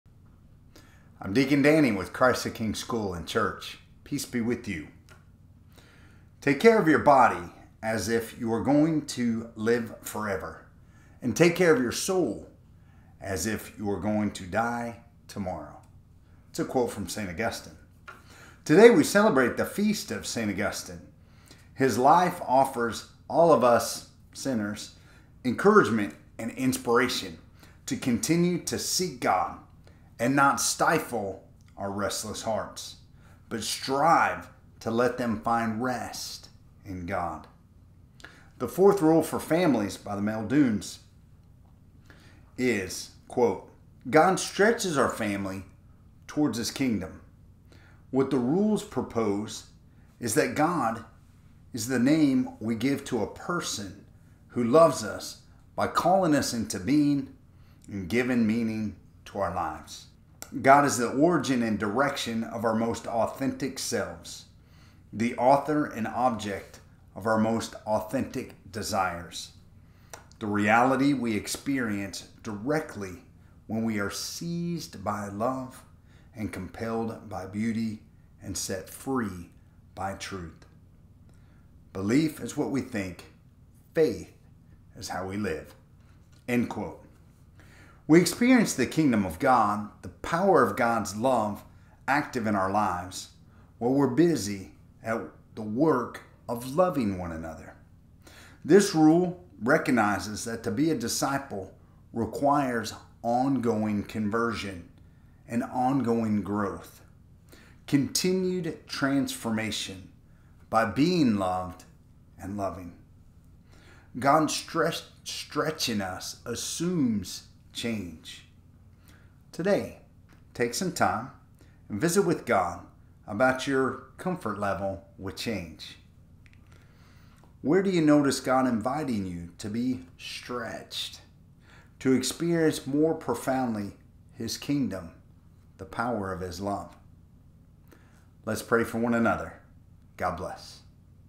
Morning Good News | Experiencing the Power of God’s love | Dc Danny | August 28, 2023
Monday 8/28/23 Experiencing the Power of God’s love
Peace be with you. “Take care of your body as if you were going to live forever. Take care of your soul as if you were going to die tomorrow” (St. Augustine). Today we celebrate the feast of the St. Augustine. His life offers all of us sinners encouragement and inspiration to continue to seek God, to not stifle our restless hearts but strive to let them find rest in God. The fourth Sacred rule for families from Tim and Sue Muldoon speaks to this: “God stretches our family towards His kingdom. What the rules propose is that God is the name we give to a Person who loves us by calling us into being and giving meaning to our lives.  God is the origin and direction of our most authentic selves, the author and object of our most authentic desires,  the reality we experience directly when we are seized by love and compelled by beauty and set free by truth. Belief is what we think; faith is how we live.” We experience the Kingdom of God, the power of God’s love active in our lives, “while we are busy at the work of loving one another”. This rule recognizes that to be a disciple requires ongoing conversion and ongoing growth, continued transformation by being loved and loving. God stretching us assumes change. Today take some time to visit with God about your comfort level with change. Where do you notice God inviting you to be stretched to experience more profoundly His Kingdom, the power of His love?  Let us pray for one another. God bless.
St. Augustine - pray for us.